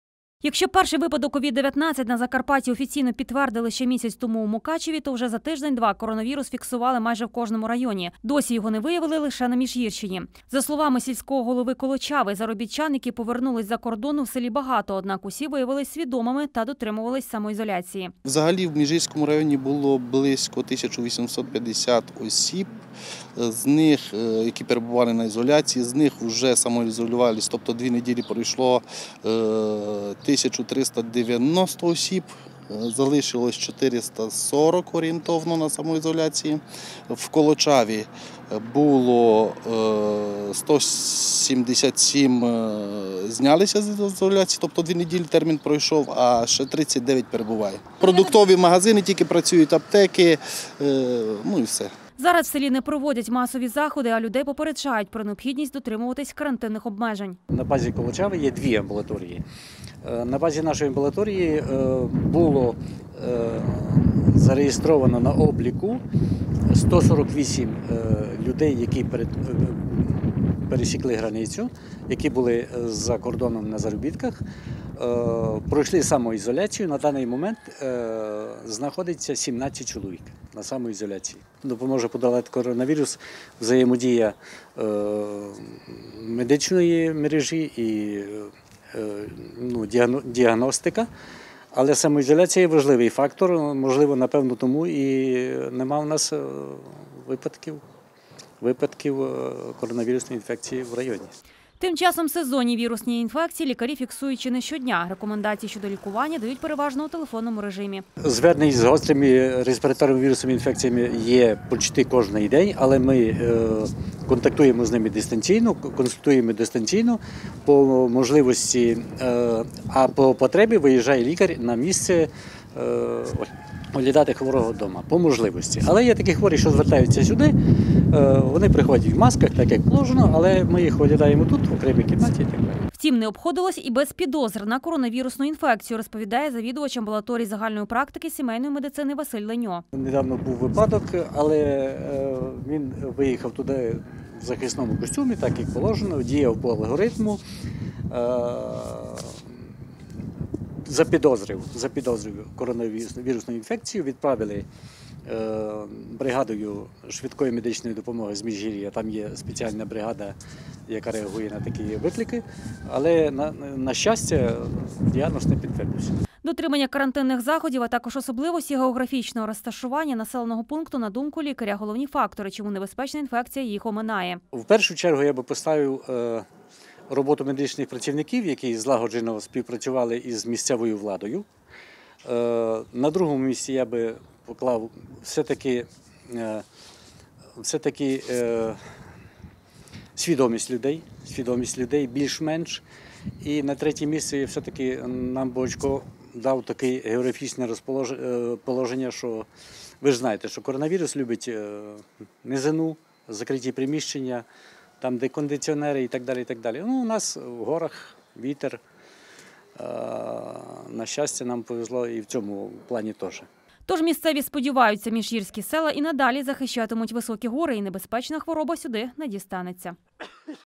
Захищені горами?
Міжгірський – єдиний район Закарпаття, де поки не виявили жодного підтвердженого випадку COVID -19. Так, у селі Колочава, понад 2 сотні людей, які повернулися з-за кордону перебували на самоізоліції, ще  в 40-ка осіб він вже завершується. Якщо перший  випадок COVID -19  на Закарпатті офіційно  підтвердили ще місяць тому у Мукачеві,  то вже за тиждень-два коронавірус фіксували майже в кожному районі.  Досі   його не виявили лише на Міжгірщині. За словами сільського голови Колочави, заробітчан, які повернулись з-за кордону  в селі багато, однак , усі виявились свідомими  та  дотримувались самоізоляції. Зараз в селі не проводять масові заходи, а людей попереджають про необхідність дотримуватись карантинних обмежень. Тим часом,  сезонні вірусні інфекції лікарі фіксують чи не щодня. Рекомендації щодо лікування дають переважно у телефонному режимі. Втім, не обходилось  і без підозр на коронавірусну інфекцію, розповідає завідувач  АЗПСМ Василь Леньо. Дотримання карантинних заходів а також особливості географічного розташування населеного пункту, на думку лікаря, - головні фактори, чому небезпечна інфекція  їх оминає. Тож, місцеві сподіваються, міжгірські села і надалі захищатимуть високі гори, і небезпечна хвороба сюди не дістанеться.